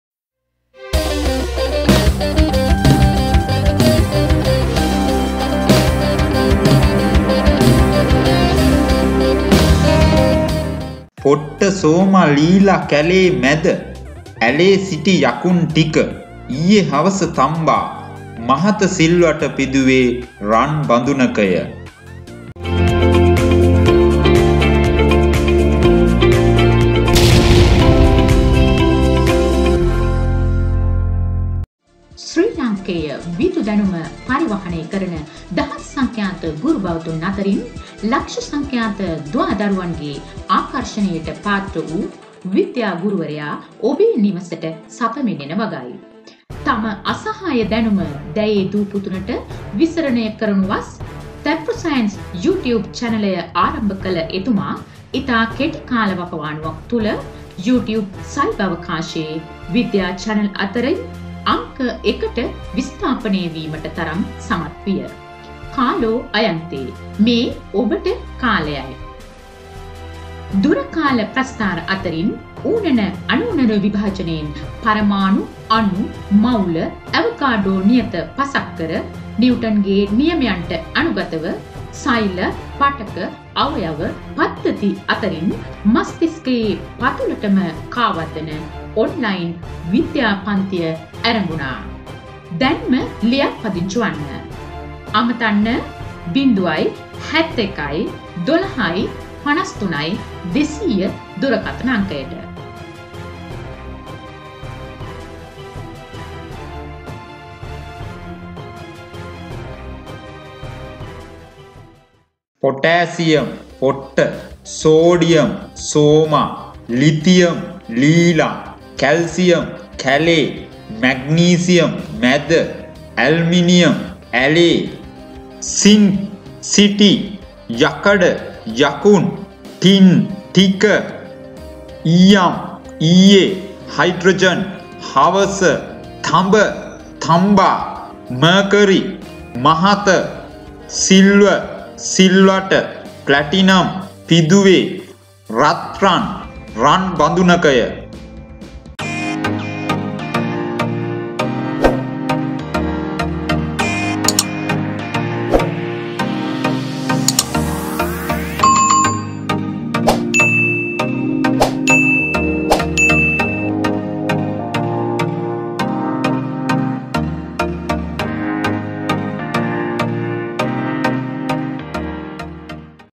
Grade 11 Science | Grade 10 Science | OL | සක්‍රියතා ශ්‍රේණිය | Sakriyata Sreṇiya | Activity Series
sakriyatā śrēṇiya|Activity Series|සක්‍රියතා ශ්‍රේණිය|

  K  -potassium (පොට්ට)
 Na  -sodium (සෝමා)
 Li -Lithium(ලීලා)
 Ca -Calcium (කැලේ)
 Mg  -magnesium (මැද)
 Al -Aluminum (ඇලේ)
 Zn -Zinc (සිටි)
 Fe -Iron (යකුන්)
 Sn -tin (ටික)
 Pb -Lead (ඊයේ )
 H -Hydrogen (හවස)
               (හයිඩ්‍රජන් ලෝහයක් නොවේ.සන්සන්දනය සදහා පමණක් භාවිතා කරයි)
 Cu -copper (තම්බා)
 Hg -Mercury (මහත)
 Ag -Sliver (සිල්වාට)
 Pt -Platinum (පිදුවේ)
 Au -Gold (රන් බදුනකය)

11 ශ්‍රේණියට අදාල අනෙකුත් වීඩියෝ නැරඔිමට පහත link භාවිතා කරන්න

Like එකක් දාන්න අමතක කරන්න එපා.👍👍👍